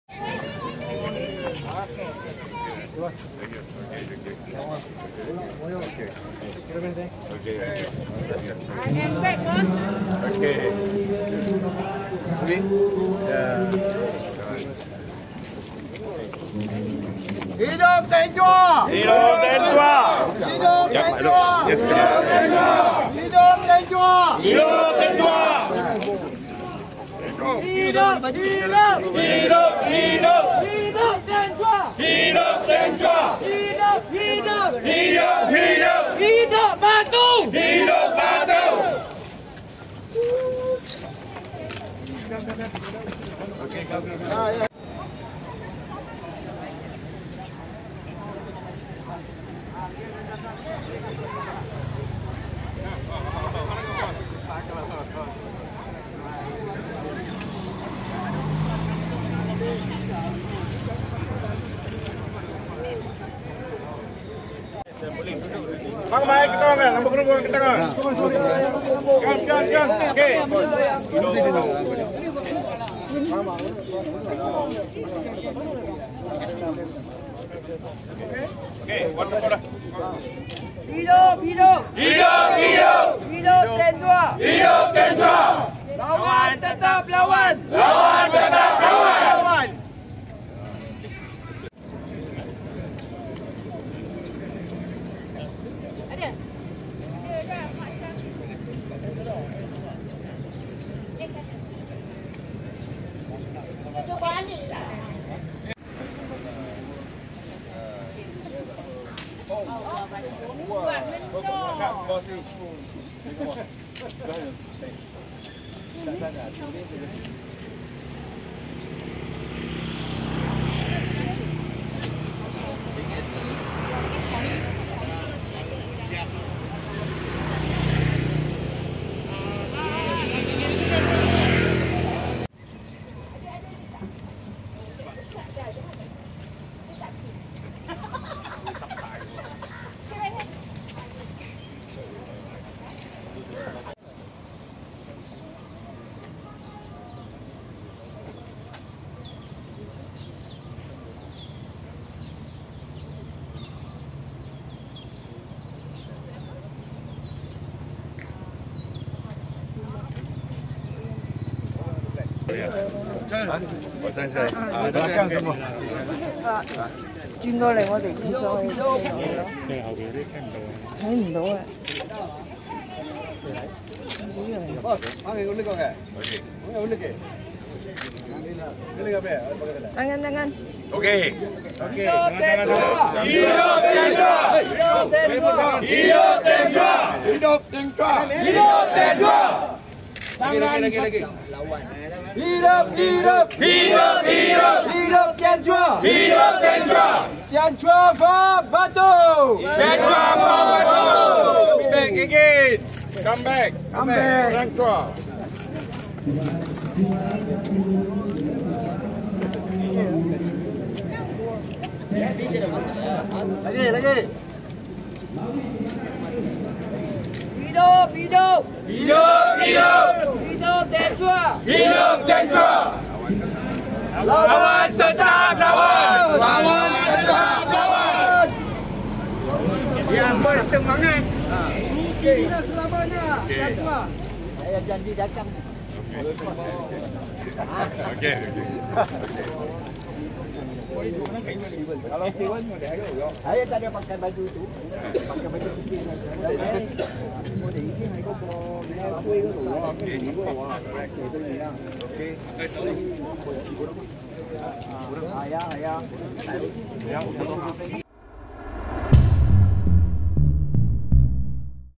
8.00am: Calon bebas Batu, Tian Chua tiba dengan 60 penyokong
Bekas ahli Parlimen Batu, Tian Chua tiba di pusat penamaan calon sebagai calon bebas dengan diiringi kira-kira 60 penyokong yang memakai kemeja T berslogan 'I'm Back'.

Tian yang tidak dicalonkan PKR bakal berdepan beberapa calon, antara lain calon partinya P. Prabakaran dan calon bebas Siti Kasim.